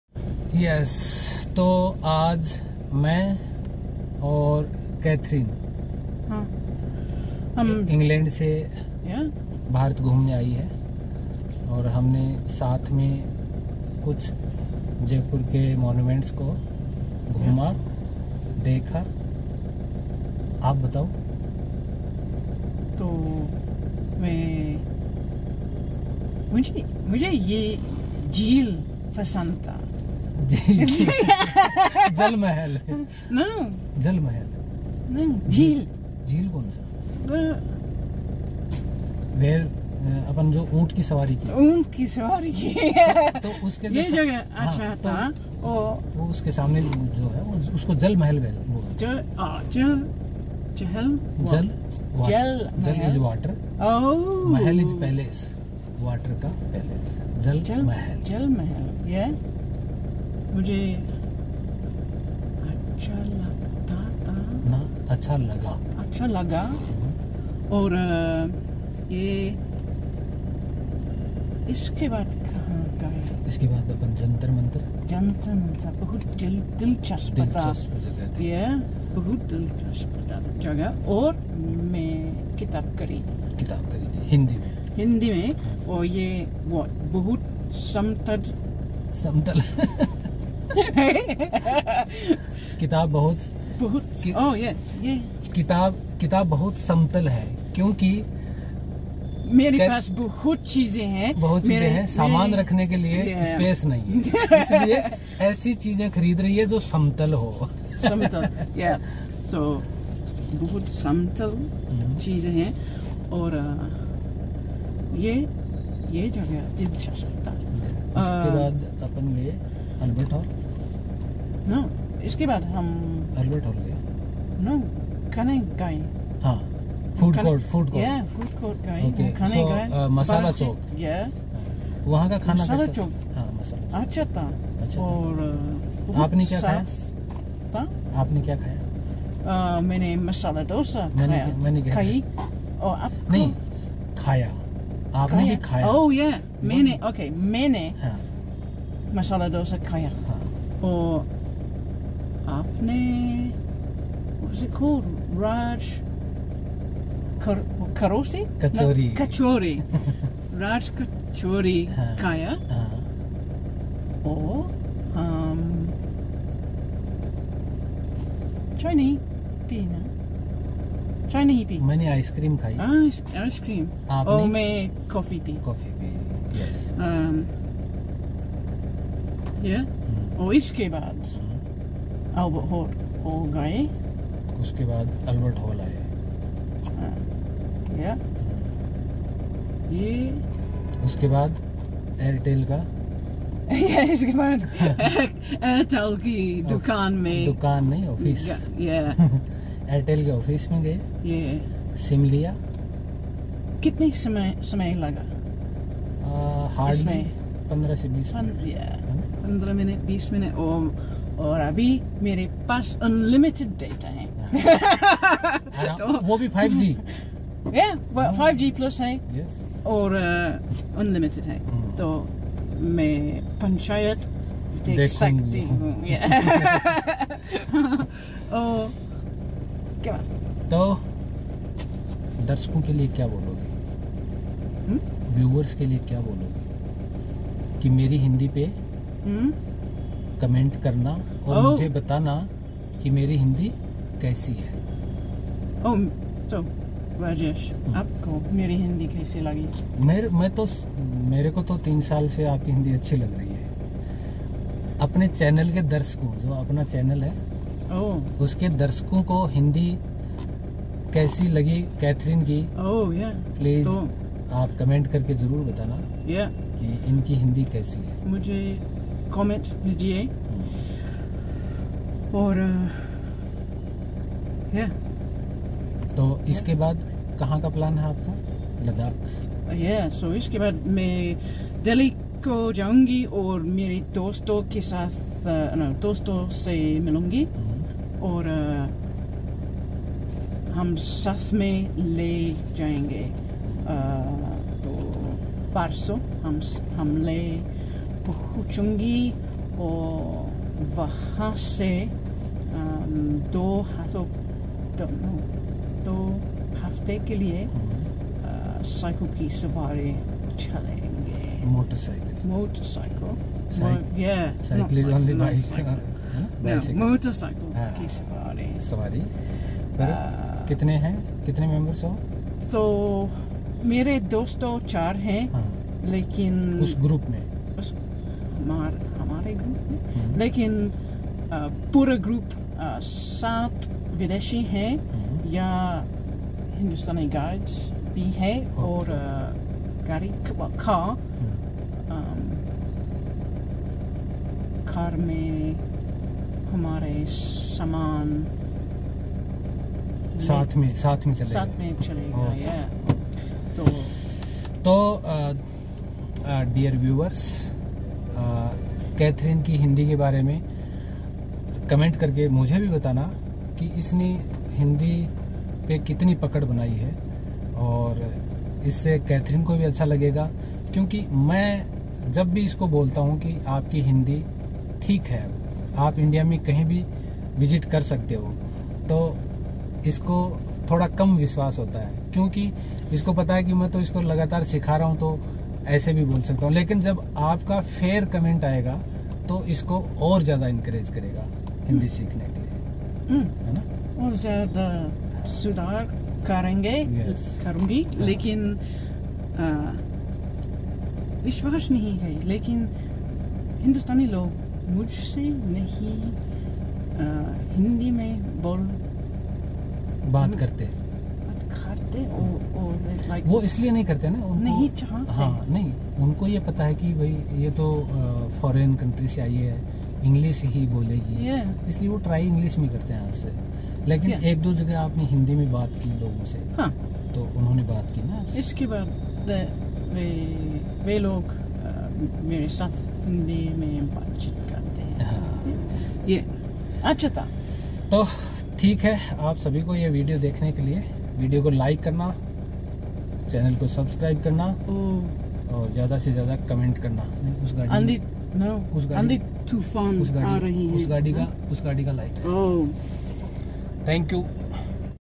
British Student Explores Jaipur: Camel Rides, Hindi Practice & Street Food Adventure!
Join us on an exciting adventure through Jaipur as we explore some of the city's most iconic landmarks! Today, I'm accompanied by Katherine, one of my students from the UK, who has been learning Hindi with me. Together, we visit the majestic Jantar Mantar, indulge in delicious Rajasthani street food at Masala Chauk, admire the stunning architecture of Albert Hall, enjoy a thrilling camel ride at the beautiful Jal Mahal, and discover many more hidden gems of Jaipur.

Throughout our journey, Katherine tries out her Hindi skills, and it’s amazing to see her enthusiasm for learning the language and exploring Indian culture.